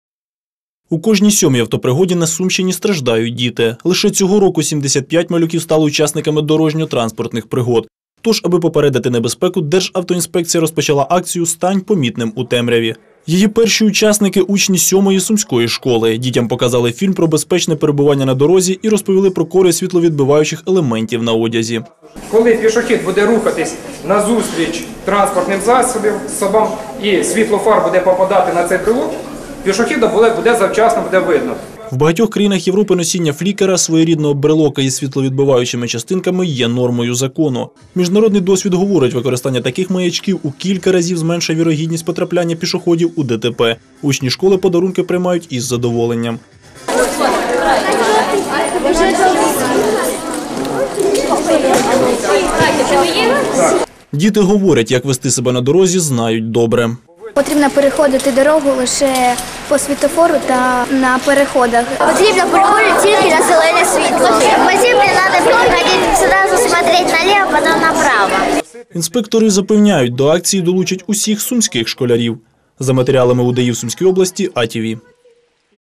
«Стань помітним у темряві»
Представники Державтоінспекції вчили сумських школярів правильному поводженню на дорозі у темну пору доби. Аби зарадити нещасним випадкам дітям подарували брелоки, що відбивають світло.